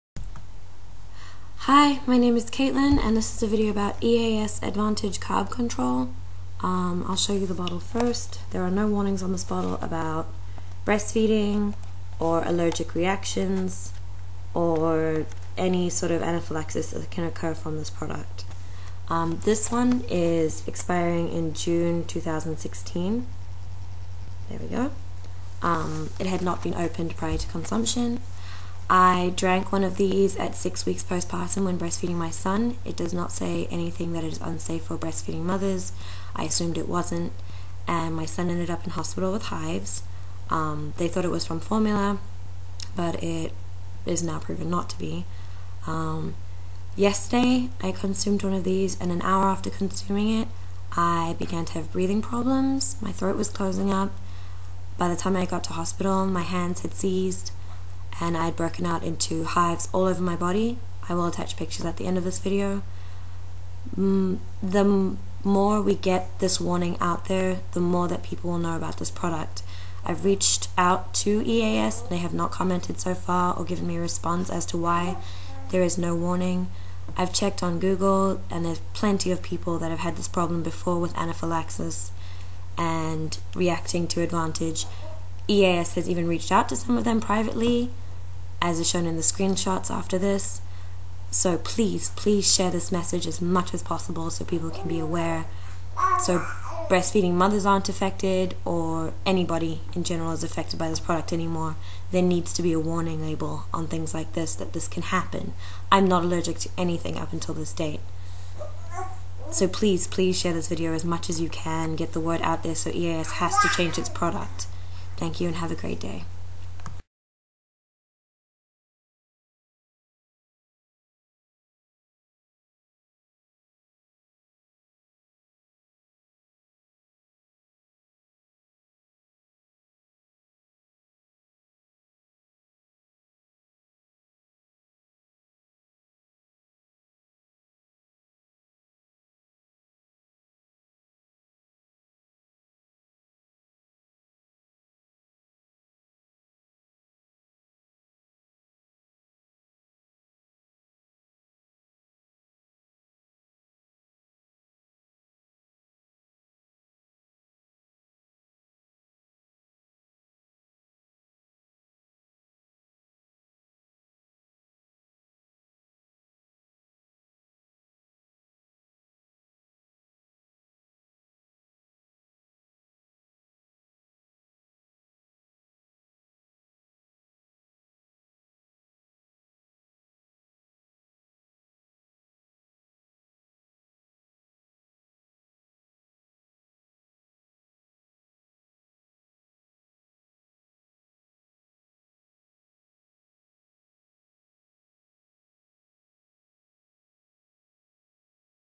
EASREACTION 0001
PLEASE SHARE. Horrible allergic reaction to EAS Advantedge low carb. Help breastfeeding moms not get affected like I did!